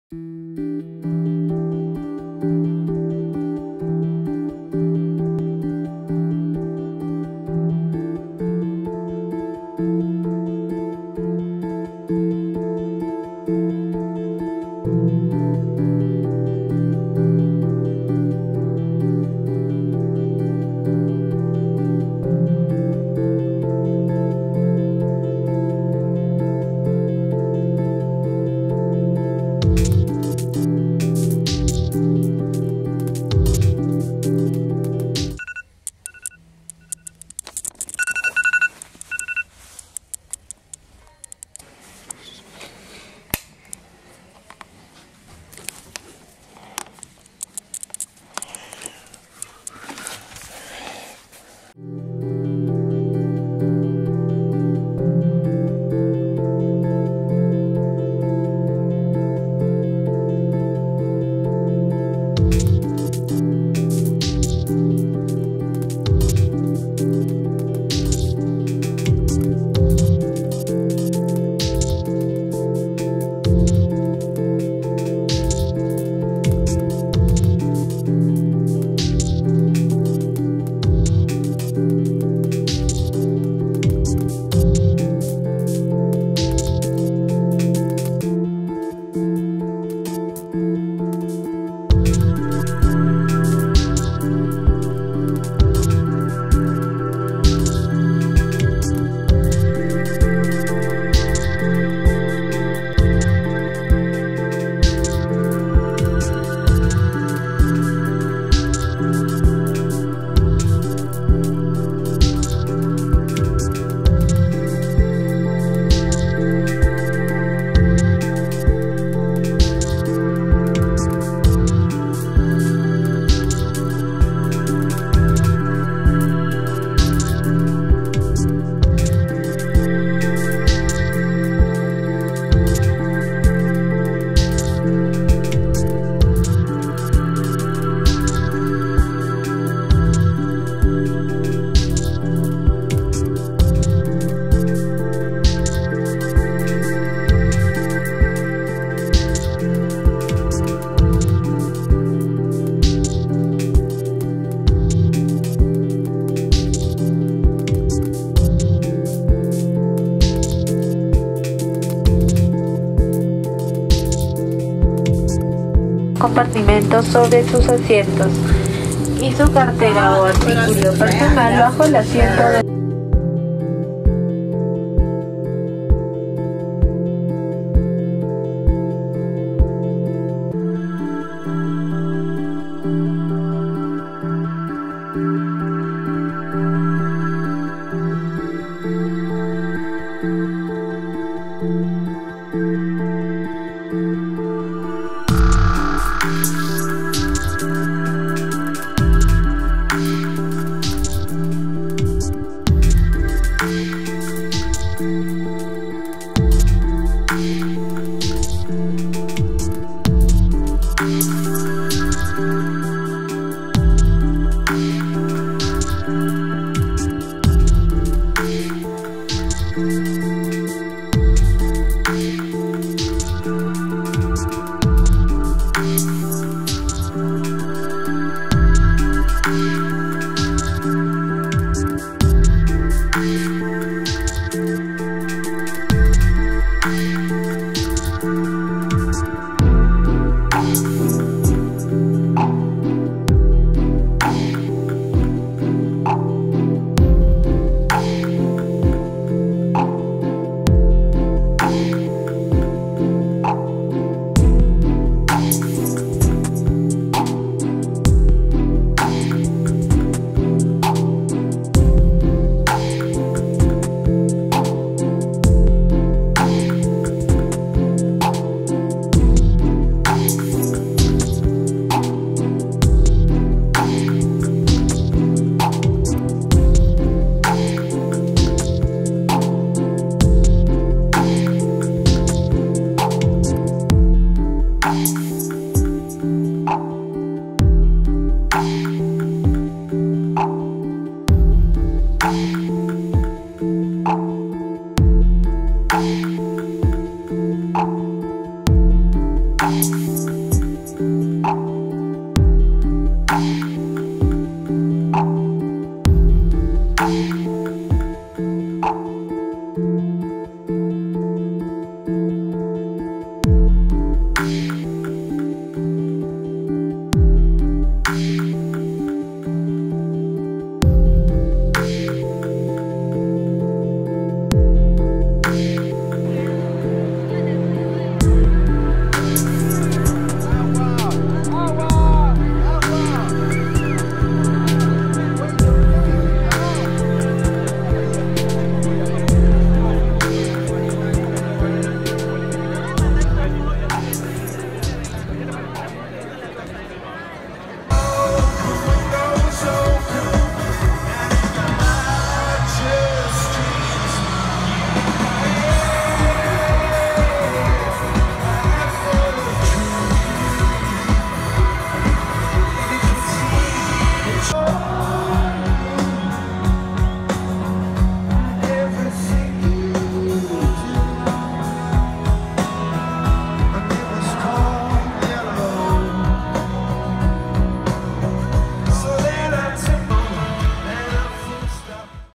Trip to Peru Day 5: Back to Lima
My time in Cusco is over and its time to slowly start heading home but first I have an overnight in Lima where the main purpose of this trip is to occur. I booked this trip to go see one of my favorite bands Coldplay so I went to see them during the AFHOD tour in Lima before heading back home.